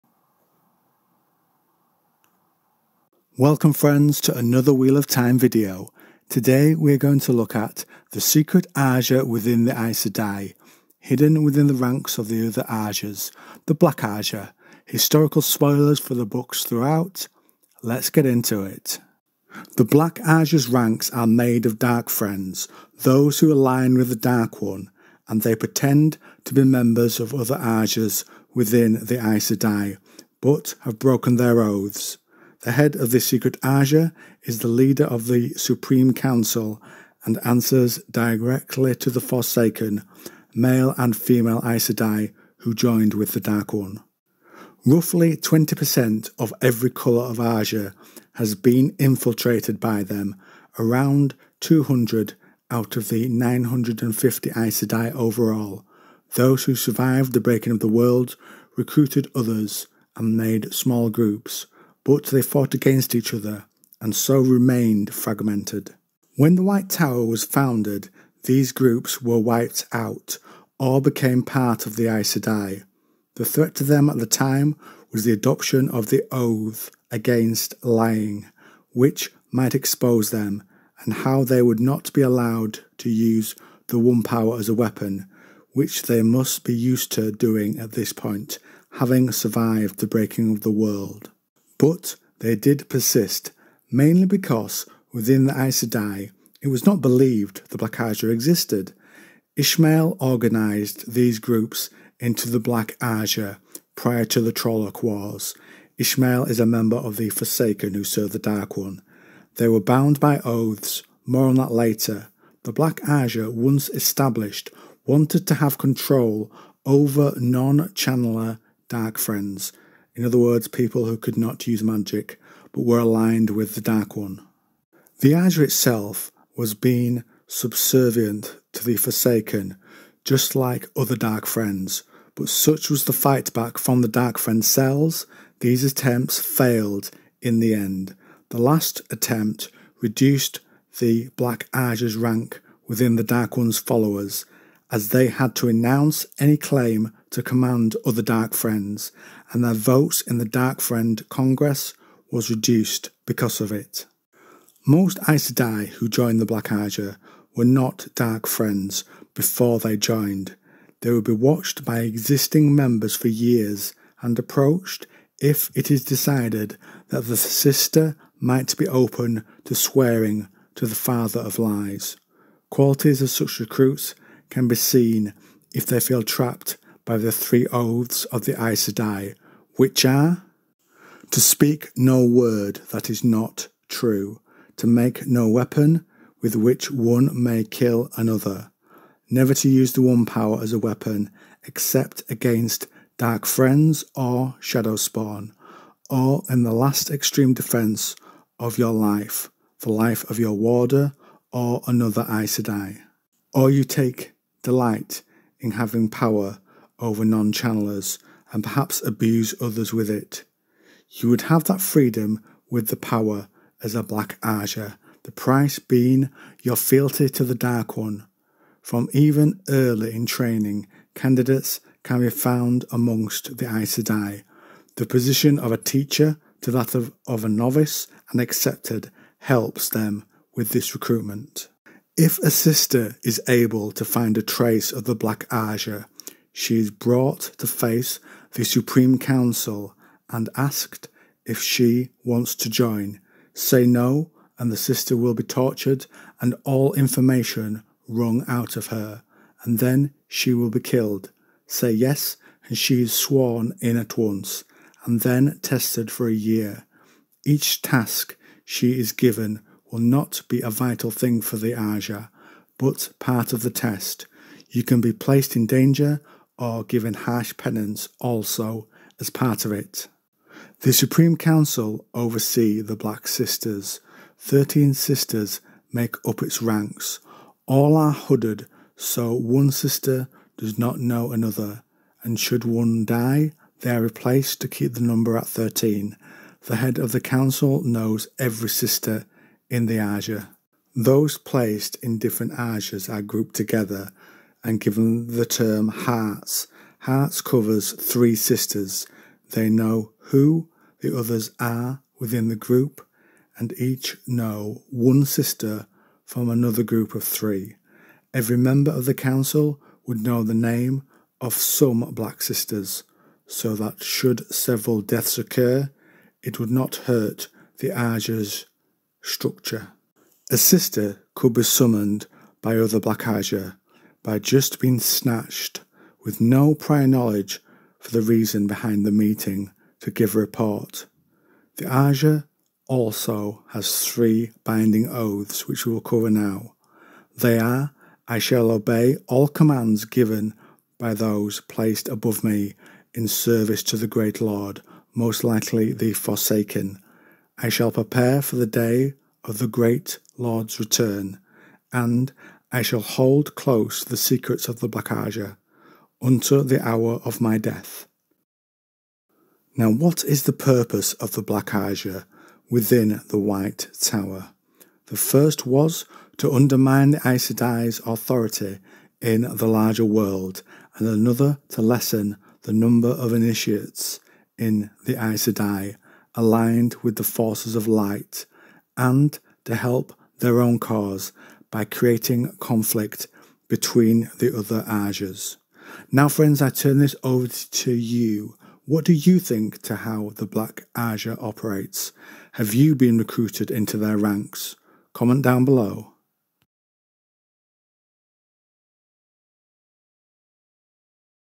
Black Ajah: The Secret Ajah Within the Aes Sedai
Welcome Friends to another Wheel of Time video, today we are going to look at the Secret Ajah within the Aes Sedai hidden within the ranks of the other Ajahs, the Black Ajah. Historical spoilers for the books throughout lets get into it!

FYI Re-release better audio

Dune Playlist Wheel of Time Playlist Lord Of The Rings Playlist Magic Weapons Playlist Grishaverse Playlist